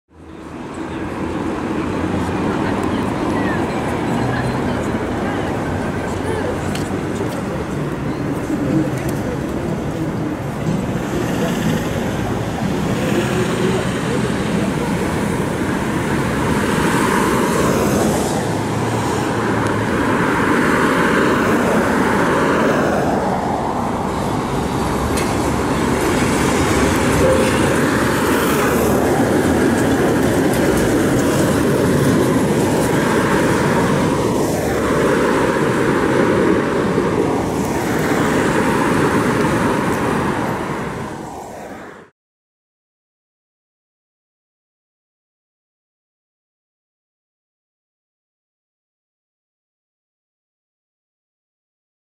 Трамвай Tatra-t3 (МТТЕ) №30125 с маршрутом №39 "Метро Чистые Пруды - Метро Университет"
Трамвай Tatra-t3 (МТТЕ) №30125 (обслуживается Трамвайным депо Имени Апакова, ГУП Мосгортранс) с маршрутом №39 "Метро Чистые Пруды - Метро Университет".

Снято в городе Москва, район Академический, Улица Вавилова. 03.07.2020 г.